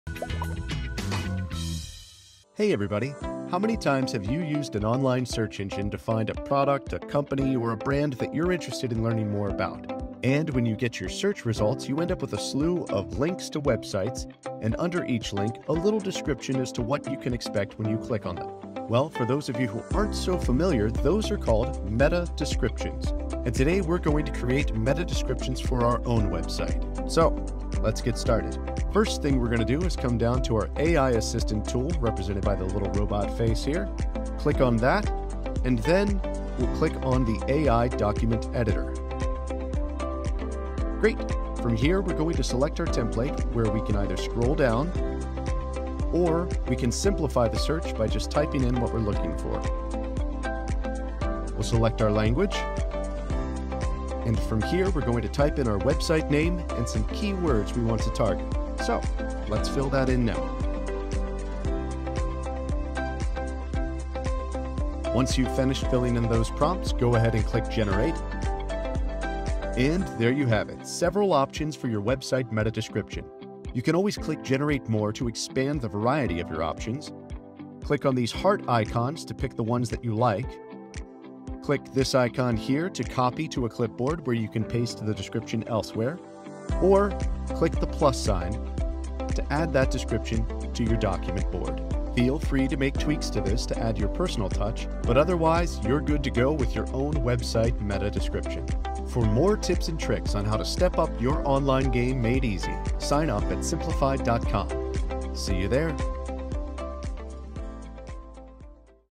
Use AI to Generate Website Metadata to Improve Visibility
Artificial intelligence can play a big role in improving your website's visibility. In this video, you'll learn how to use AI to generate metadata for your website that will help it rank higher in search engine results pages.

☑ Generate Website Metadata
☑ Improve Visibility
☑ AI Writer
☑ Ranking Metadata
☑ AI